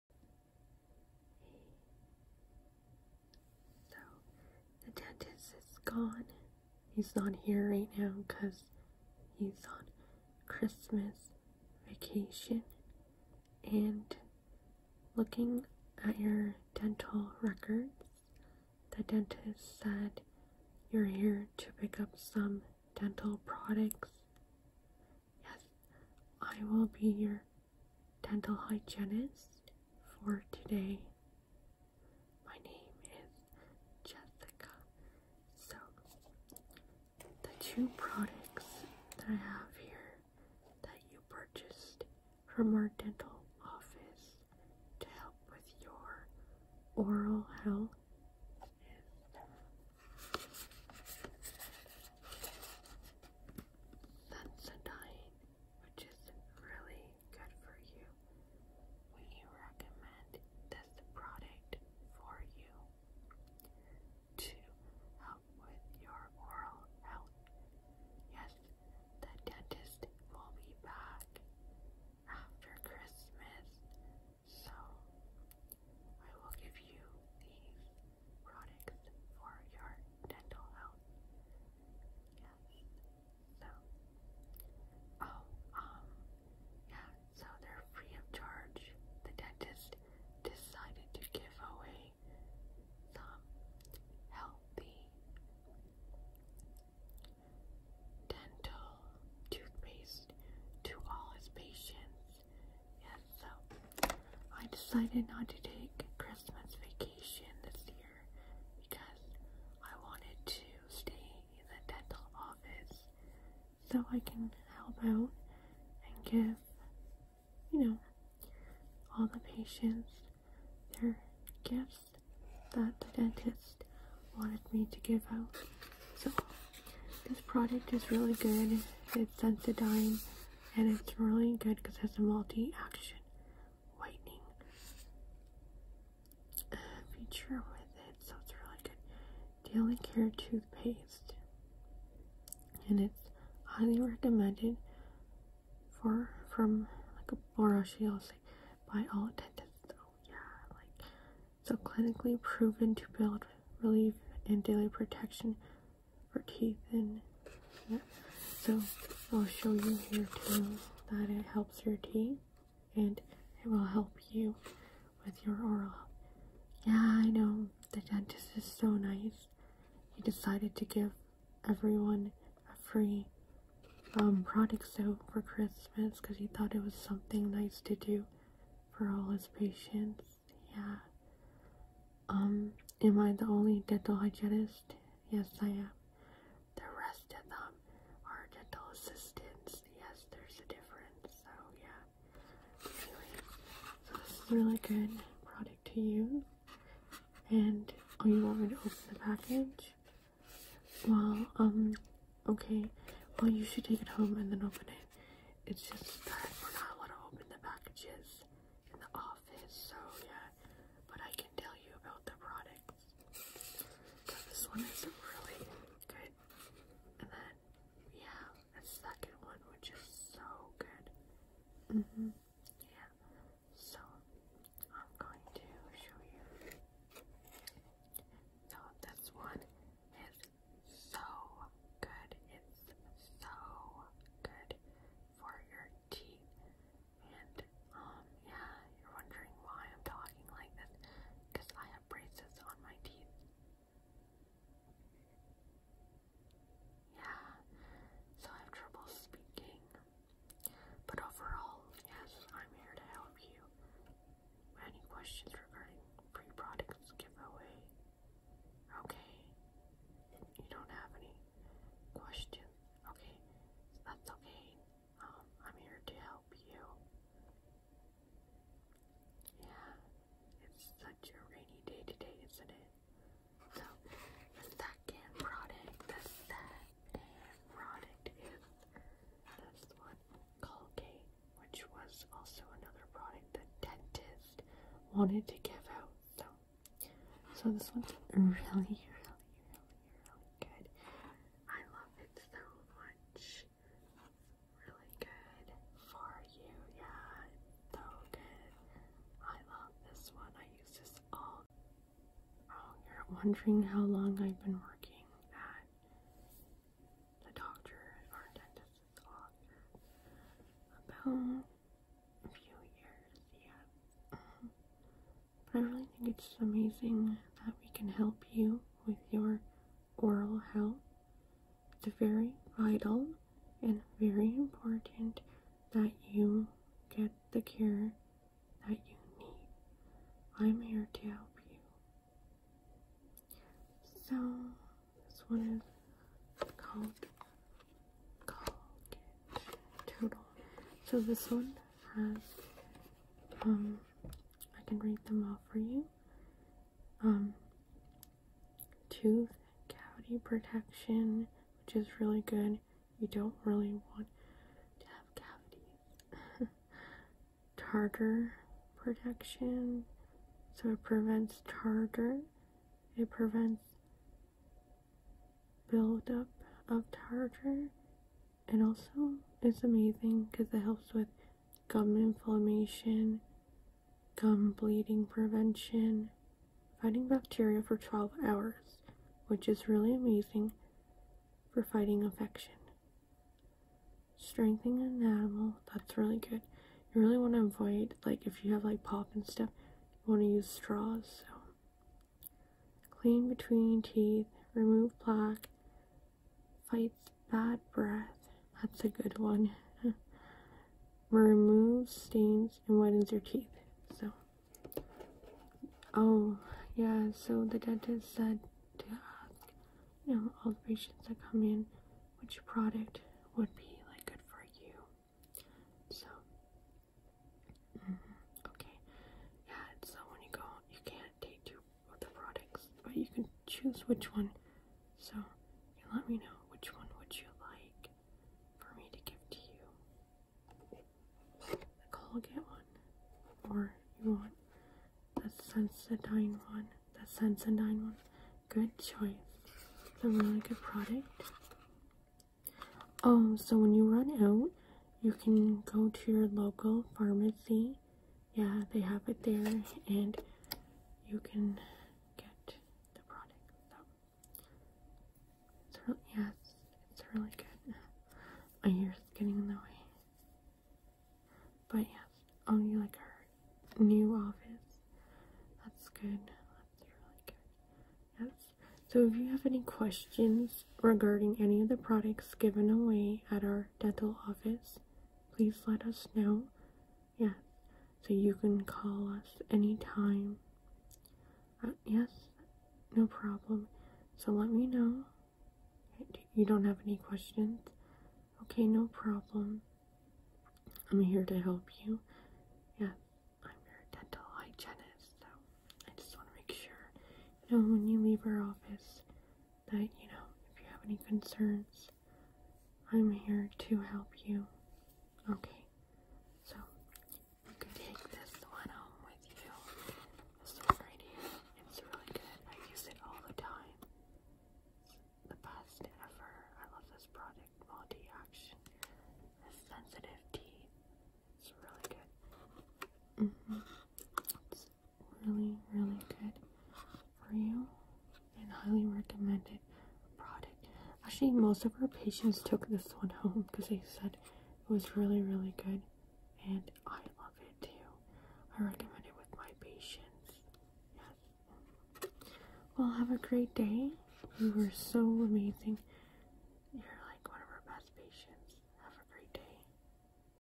ASMR Dental Hygienist Whispering Roleplay
Hello everyone, welcome back once more to Jessica ASMR!  In today's video we will be doing an ASMR Dental Hygienist Whispering Roleplay.  I am giving away some Sensodyne and Colgate toothpaste . The dentist wanted me to give our patients dental toothpaste for Christmas Enjoy.
 You teeth are going to look nice and white.

Thanks for tuning in to Jessica ASMR!  I hope you all enjoyed and have a happy holiday season!

- J